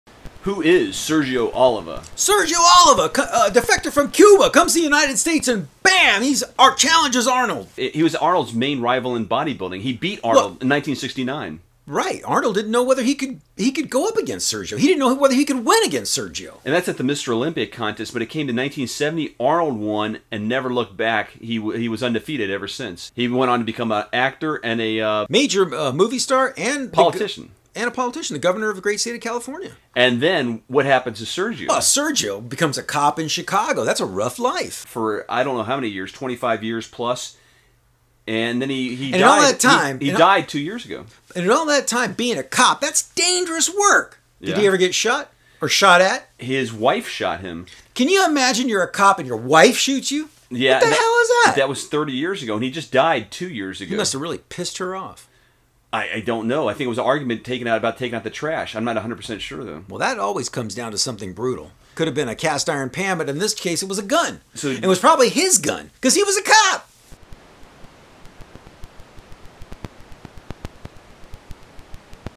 1970 Mr Olympia Discussion
The future of Arnold Schwarzenegger was dependent on defeating Sergio Oliva at the Mr. Olympia Contest.

1970 Mr. Olympia

BEGIN TRANSCRIPT

Who is Sergio Oliva?

Sergio Oliva defected from Cuba, comes to the United States and BAM! He challenges Arnold.

He's Arnold's main rival in bodybuilding. He beat Arnold in 1969.

Right. Arnold didn't know whether he could go up against Sergio. He didn't know if he could win against Sergio.

And that's at the Mr. Olympia contest. When it came to 1970, Arnold won and never looked back. He was undefeated ever since. He went on to become an actor.

Major movie star and politician, the governor of the great state of California.

And then what happened to Sergio?

Sergio becomes a cop in Chicago. That's a rough life.

For, I don't know how many years, 25 years plus, and then he died two years ago.

And in all that time being a cop, that's dangerous work. Did he ever get shot or shot at?

His wife shot him.

Can you imagine? You're a cop and your wife shoots you? What the hell is that?

That was 30 years ago and he just died two years ago.

He must have really pissed her off.

I don't know. I think it was an argument about taking out the trash. I'm not 100% sure though.

Well, that always comes down to something brutal. Could have been a cast iron pan but in this case it was a gun. It was probably his gun because he was a cop!

END TRANSCRIPT